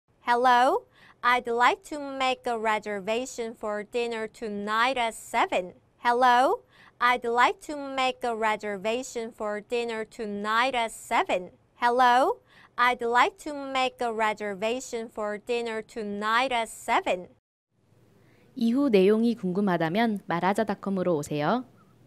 “저녁식사를 예약하려고 합니다”를 영어로? 말하자닷컴으로 배우는 진짜 상황별 영어회화
[말하자닷컴]오늘의 상황별 영어회화!
 "안녕하세요. 오늘 저녁 7시에 저녁식사 예약하려고 합니다"
→ Hello, I'd like to make a reservation for dinner tonight at 7.

해외에서 유용한 필수 영어회화
"이 버스 공항으로 가나요?"를 배우고 싶다면?